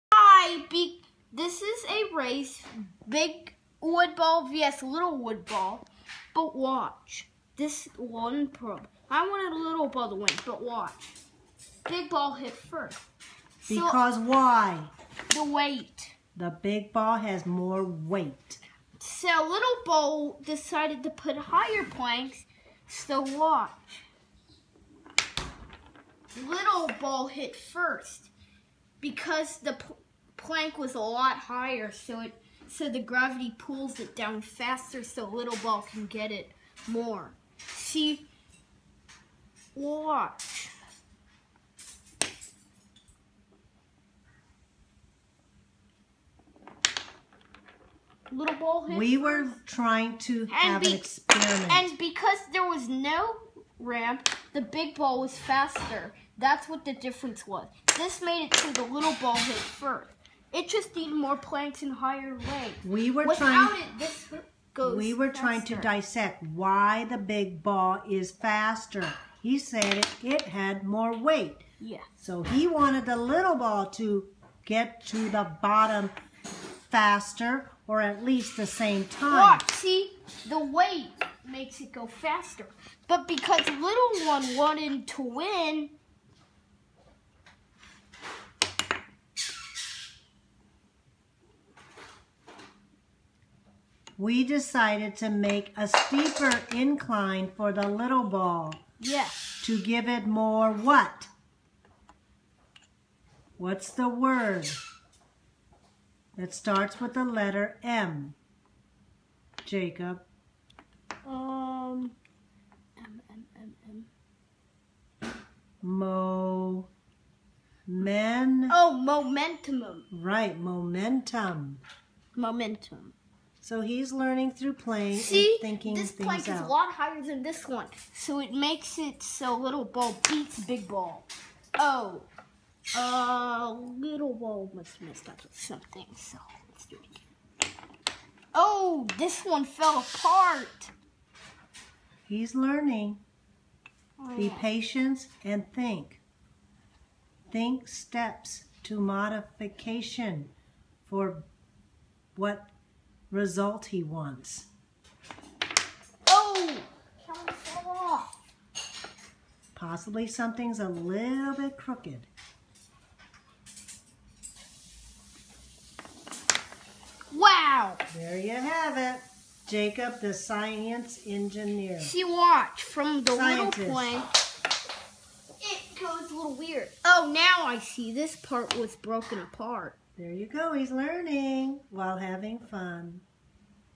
Kidjacob the engineer scientist
Big ball versus little ball both made out of wood he wants to see which ball goes down faster The big ball goes down faster so he modifies so to see if he can make a little ball go down faster or at least the same rate. He decided to make the smaller balls ramp have a steeper incline and it worked